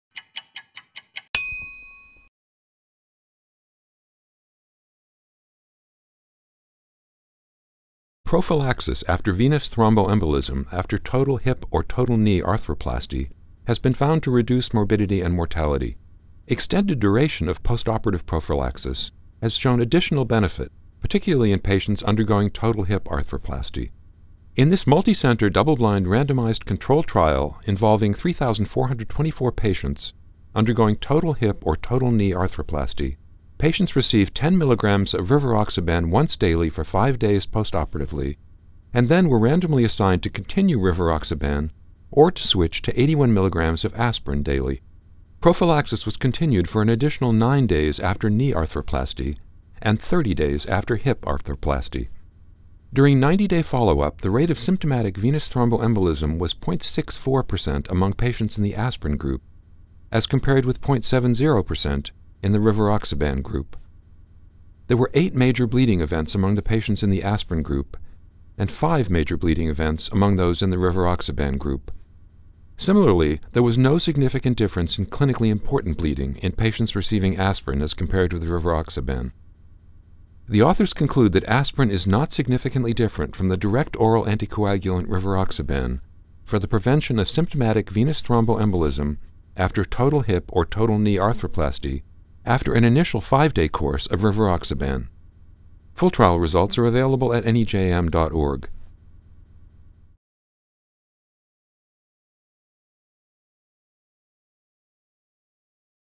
Venous Thromboembolism Prophylaxis after Hip or Knee Arthroplasty
Direct oral anticoagulants are commonly used for extended prophylaxis against venous thromboembolism after total hip or knee arthroplasty. Trials and meta-analyses have suggested that aspirin may be equally effective. Full study